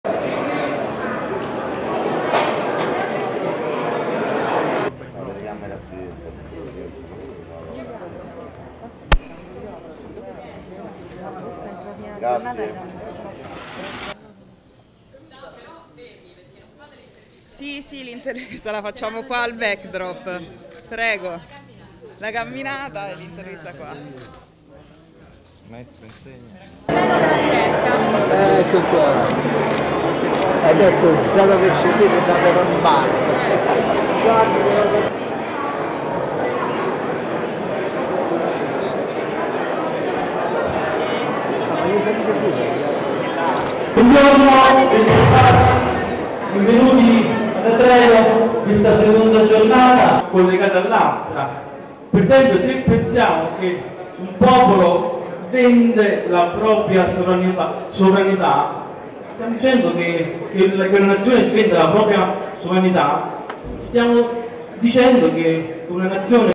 ATREJU 2015: DIBATTITO CON QUAGLIARIELLO, TOTI, ORFINI, RAMPELLI - IMMAGINI
(Agenzia VISTA) - Roma, 26 Settembre 2015 - Atreju 2015, l'officina della destra è la kermesse organizzata da Fratelli d'Italia a Roma dal 25 al 27 Settembre.